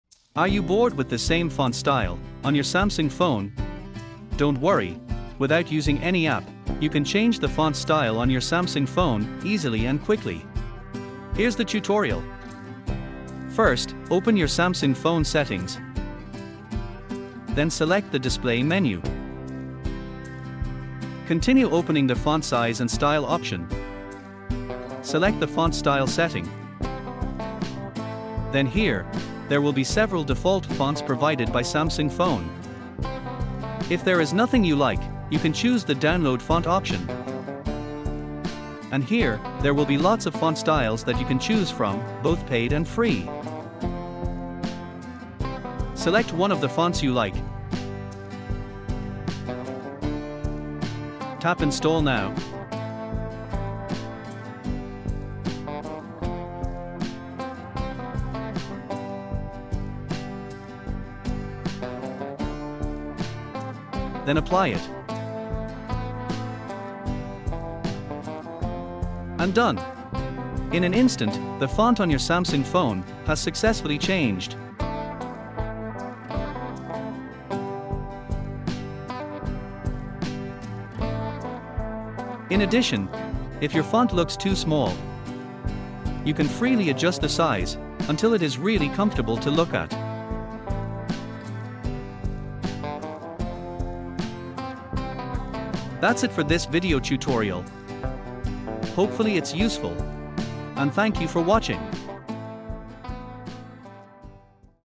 How to Change Font Style on Samsung Android Phones Without Any Apps
How to change font style in Samsung. Are you tired of the same old font style on your Samsung Android phone? Good news! You can change it without using any third-party apps. In this video, we’ll show you how to easily update your font style through the phone's settings.

Follow these simple steps:
1. Go to Settings.
2. Select Display.
3. Open Font size and style.
4. Choose Font style.

Here, you can select from a variety of pre-installed fonts or download new ones. Changing the font style will affect all aspects of your phone's display, including the Notes app, lock screen with date and time, Samsung keyboard, and more.